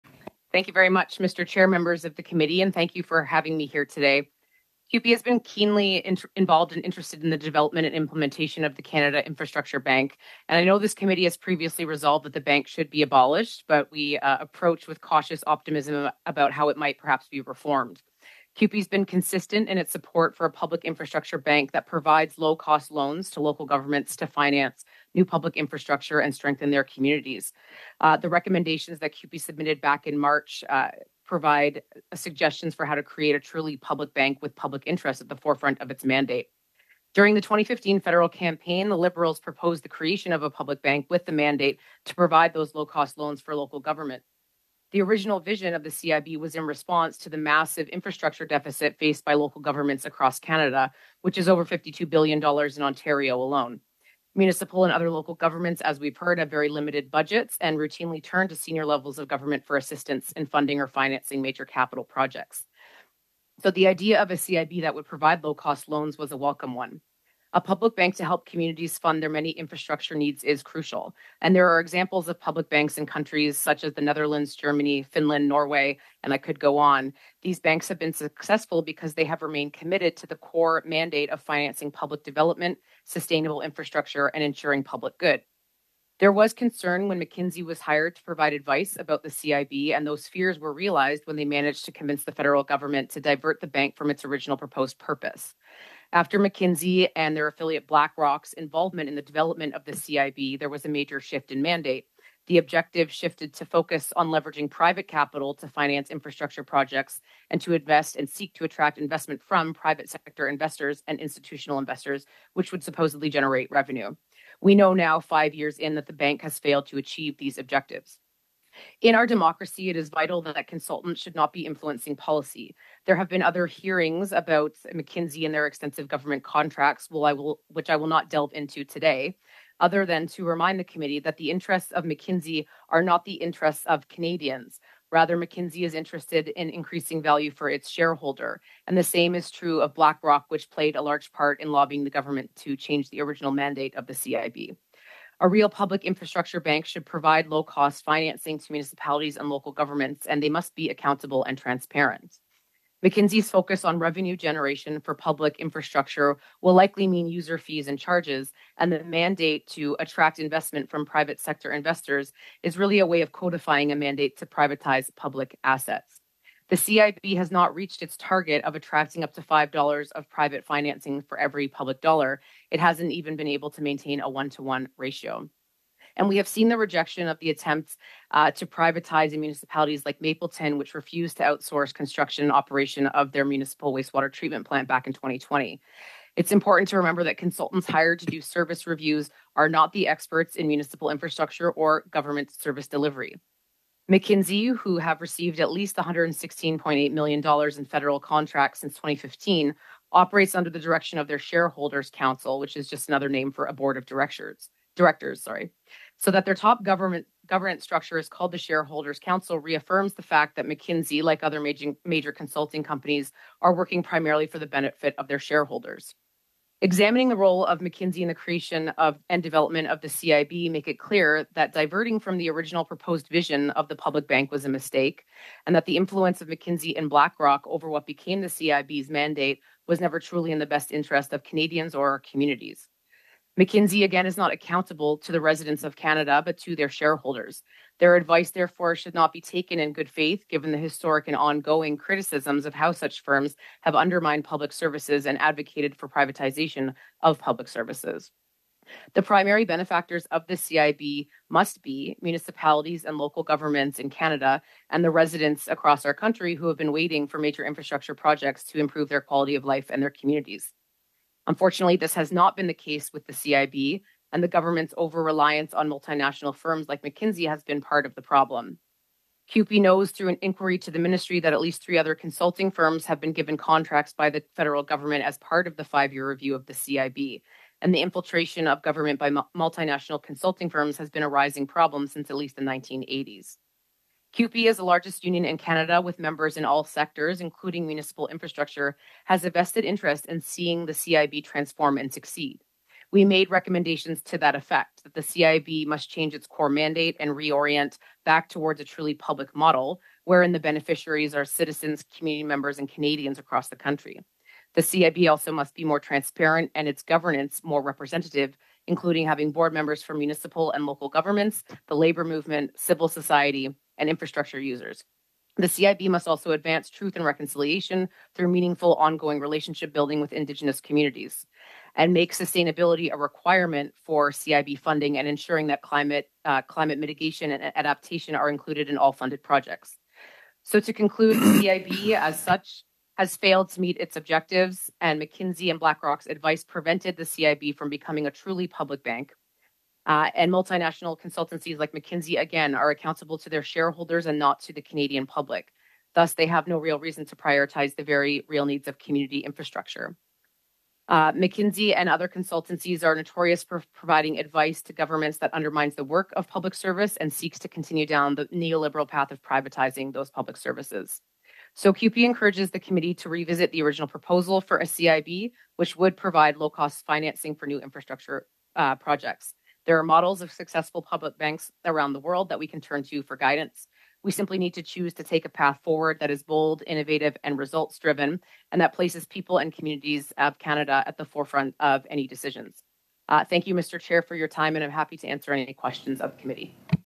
Consultants broke the infrastructure bank. Let’s rebuild a public bank.
Advice from giant consulting firm McKinsey broke the Canada Infrastructure Bank, and it’s time to fix that mistake.

On May 16, CUPE made our case for building a truly public bank at a House of Commons committee investigating the role of McKinsey in the bank’s design and early days.

Senior research officer Diane Therrien outlined how McKinsey and other corporate players like BlackRock radically shifted the bank’s mandate.